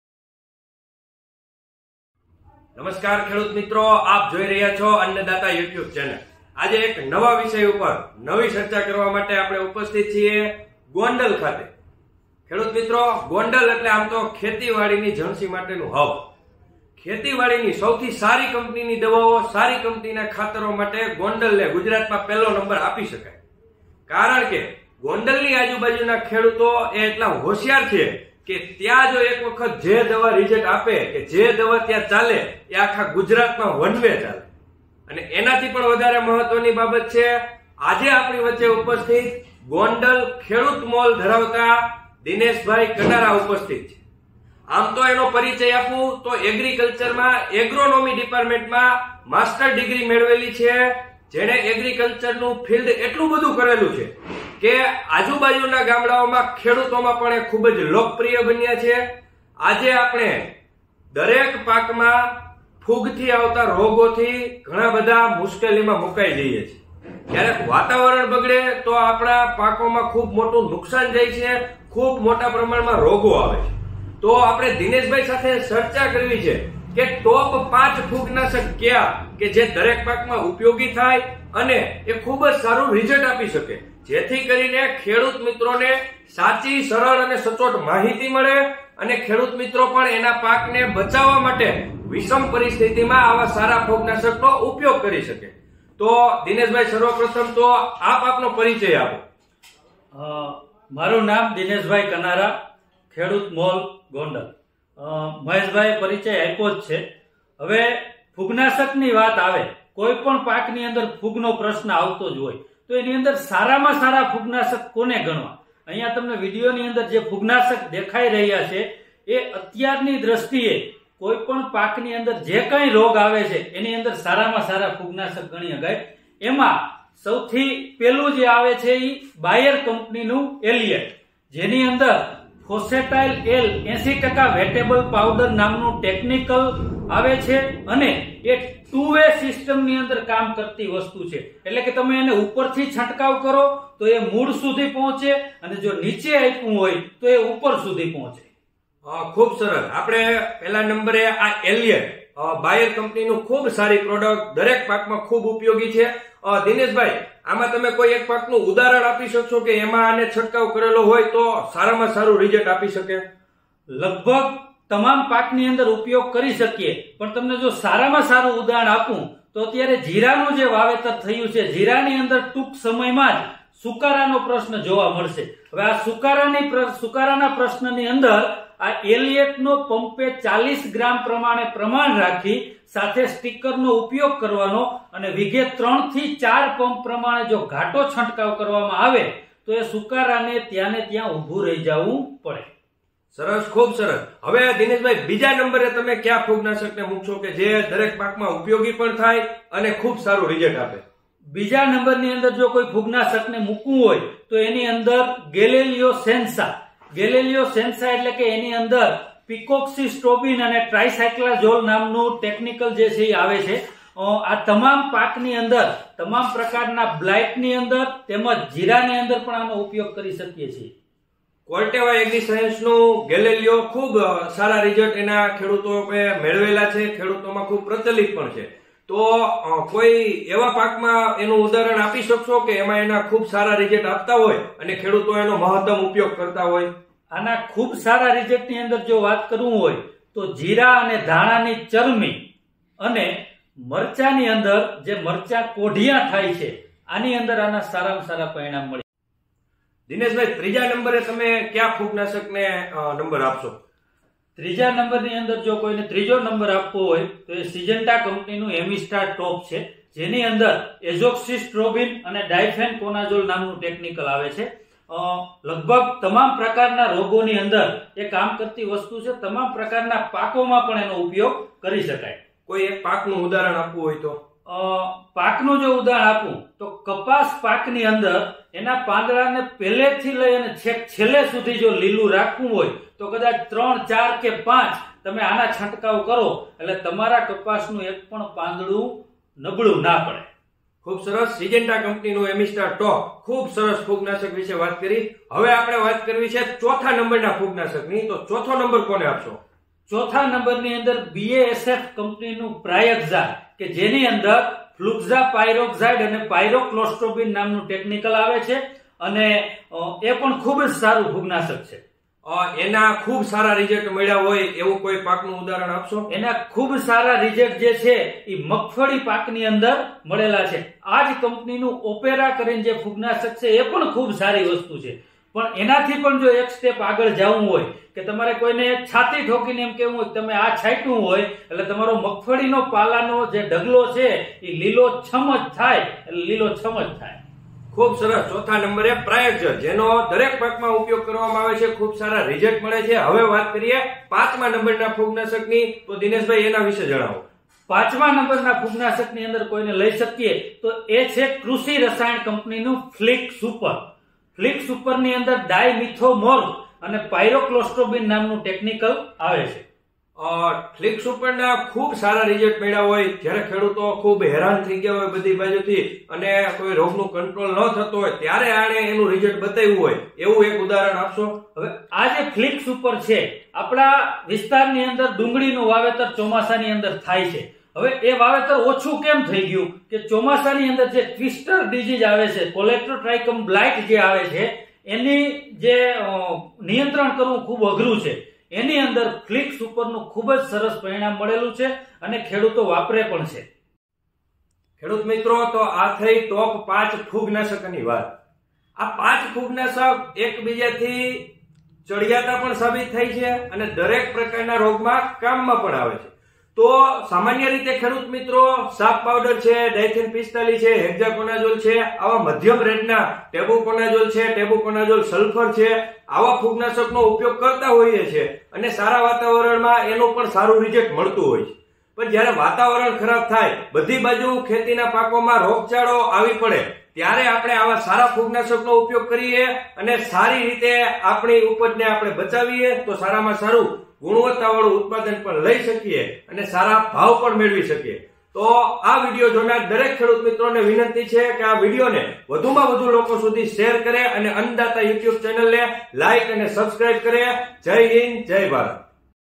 Top 5 ફુગનાશક વિશે જાણો_અન્નદાતા |8780622775| સૌથી બેસ્ટ ફૂગનાશકની વાત_પ્રગતિશીલ ખેડૂતો માટે
દરેક ખેડૂત મિત્રોને સારા ફૂગનાશકની માહિતી મળી રહે એ આશયથી આ વિડીયો બનાવ્યો છે.

TOP 5 FUNGICIDE

સૌથી બેસ્ટ ફૂગનાશકો

સરળ, સચોટ અને સાચી માહિતી